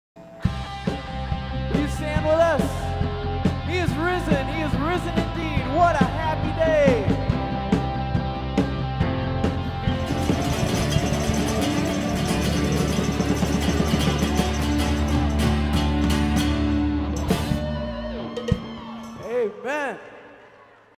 DW having FUN
A quick time lapse of DW rockin out at worship team practice.